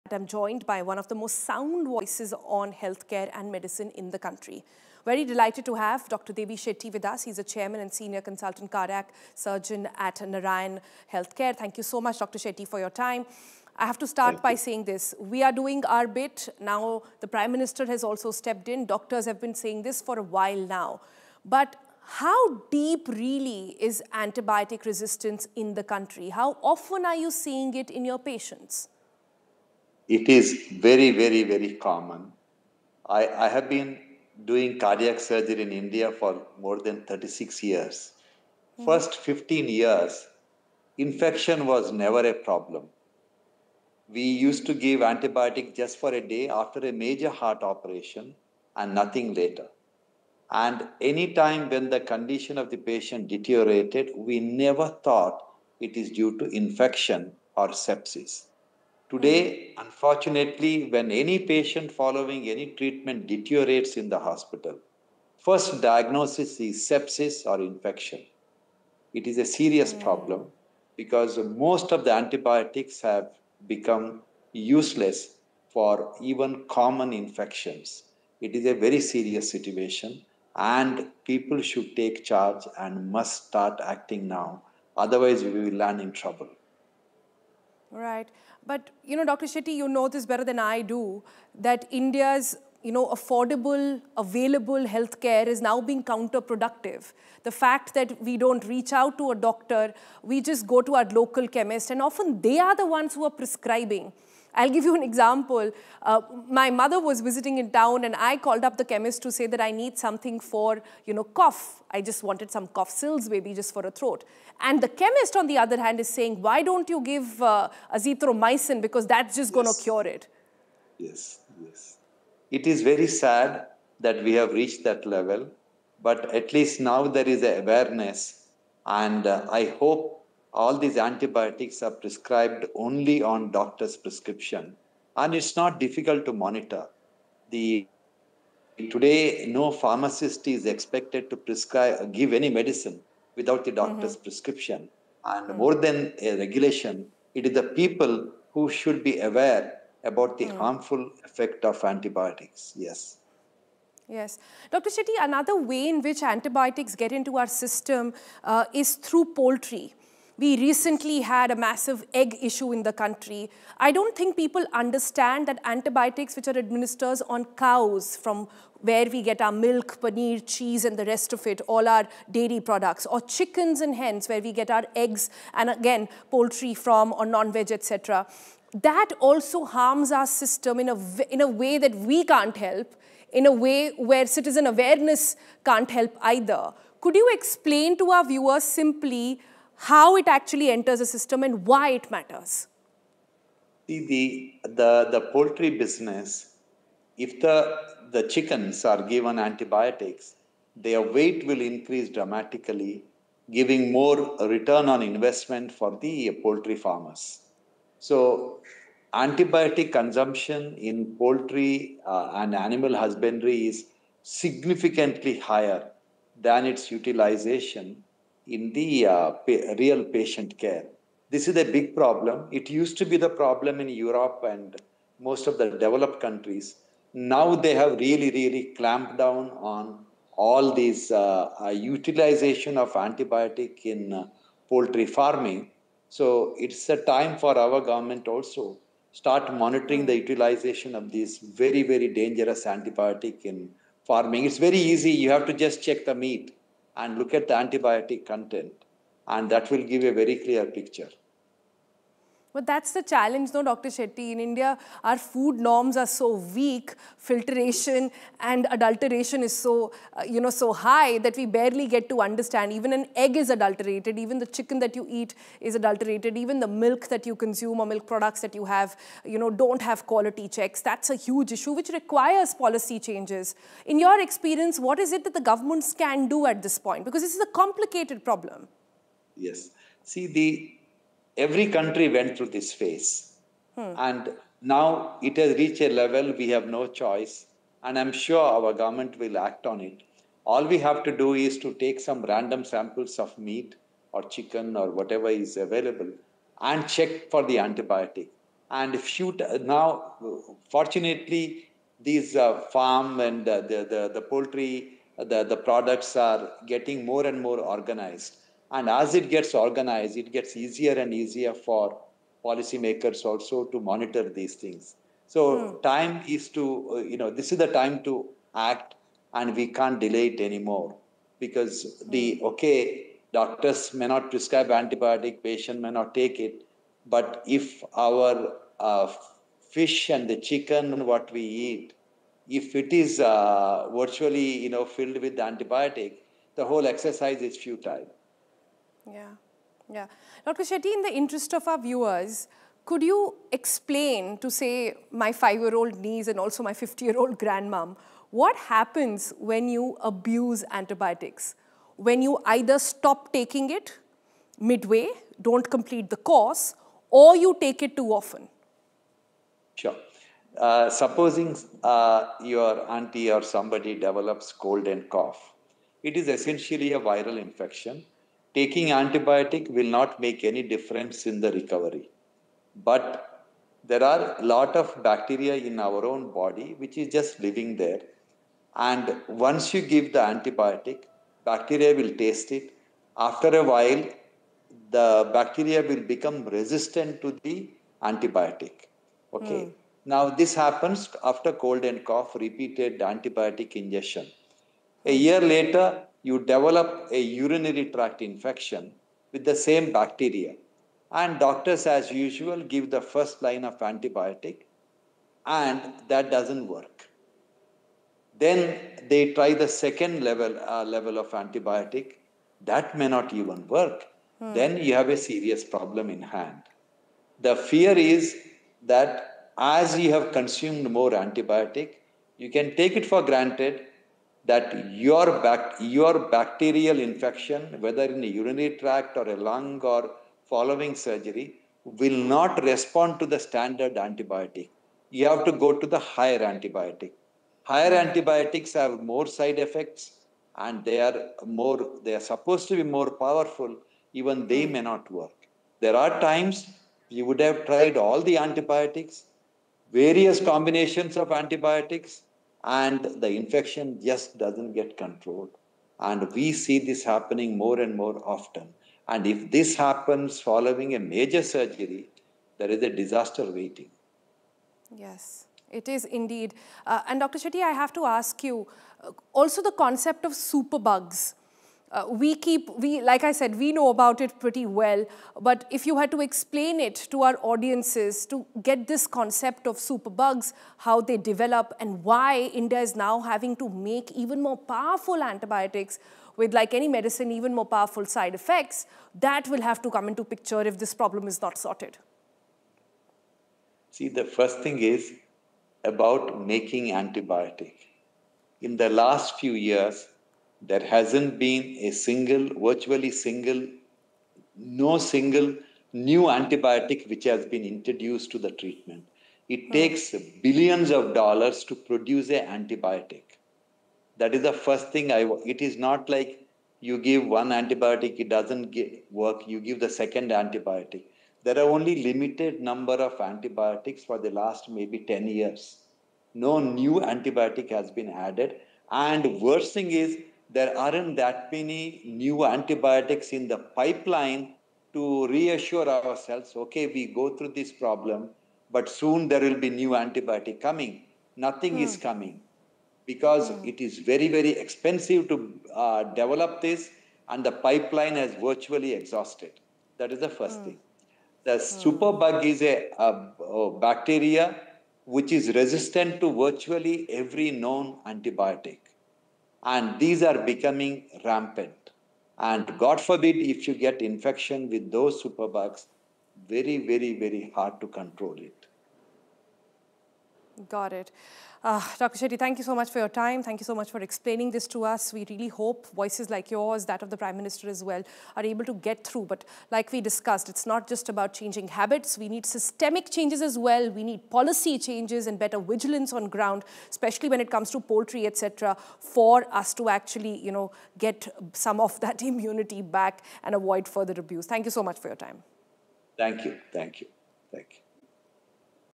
'Most Antibiotics Useless For Common Infections': Dr. Devi Shetty On Antibiotic Crisis
PM Modi warns of Antimicrobial Resistance in Mann Ki Baat. Dr Devi Shetty reveals massive antibiotic misuse in poultry & animal husbandry fueling superbugs. Overuse in self-medication threatens treatments for pneumonia, UTIs.